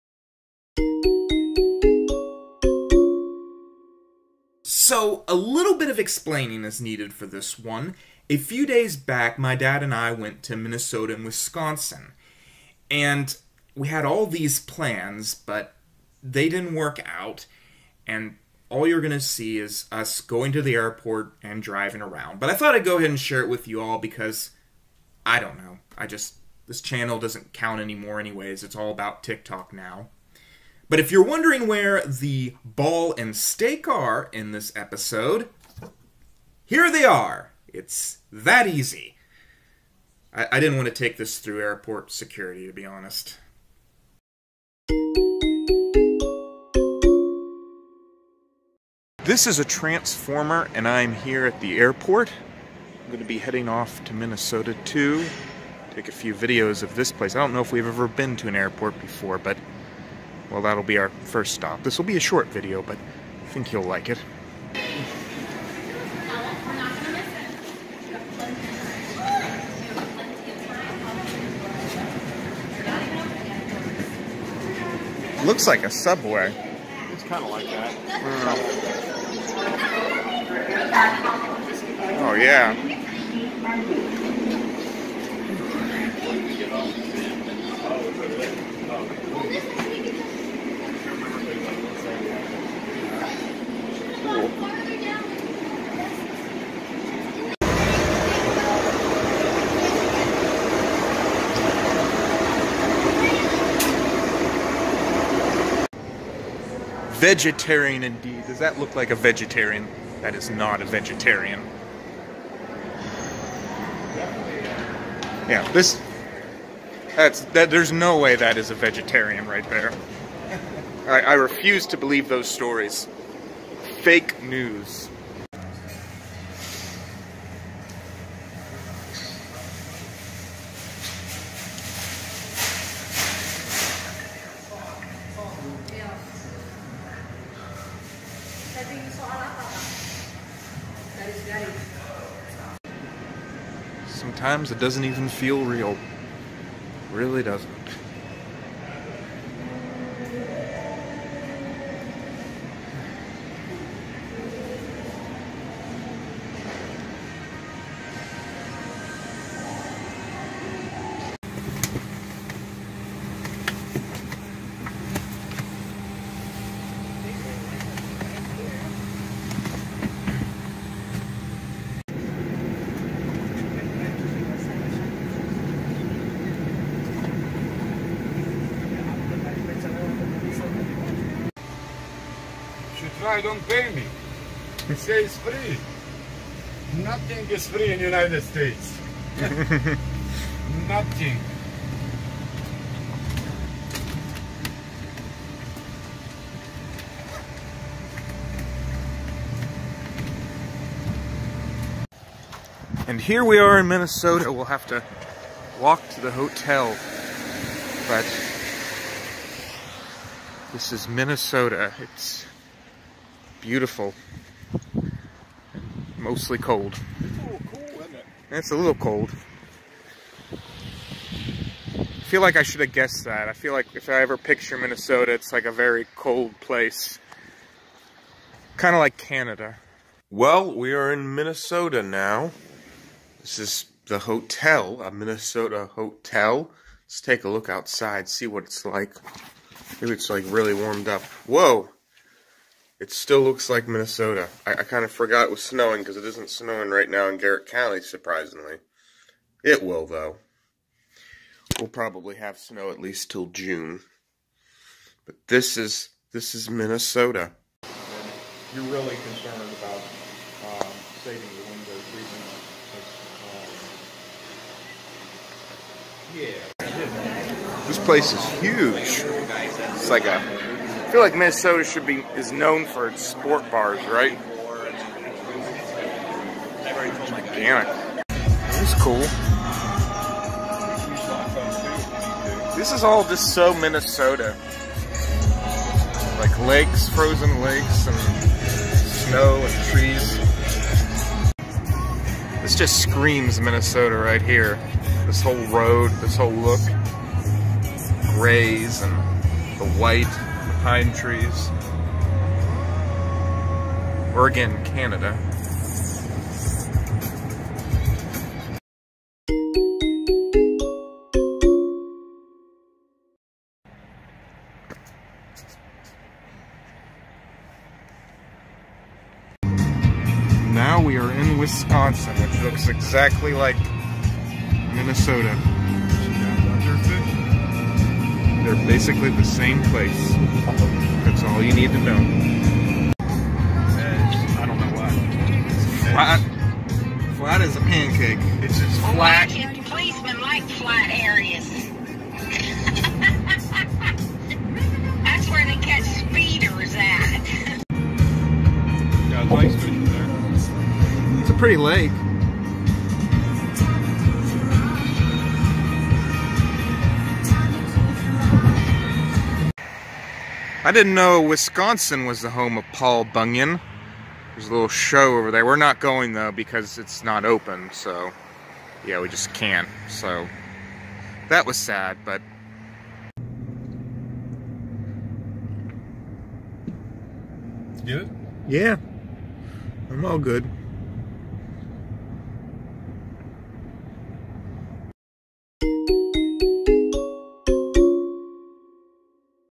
Wisconsin and Minnesota Vlog!
Episode #807. I traveled to Wisconsin and Minnesota during the weekend. Here is the story.